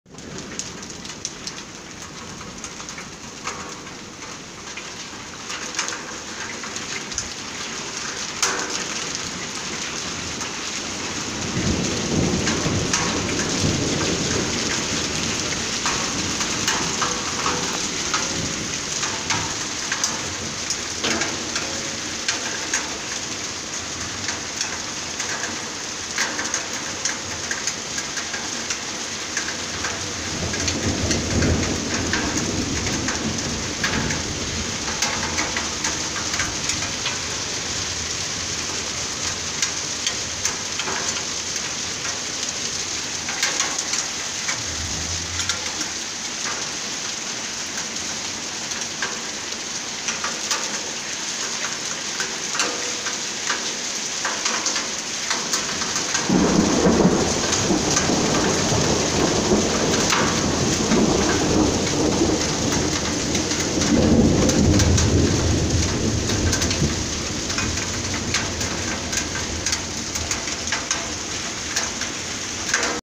Grandinata bis 11 maggio 2019 val brembilla
A poche ore dalla prima, concede il bis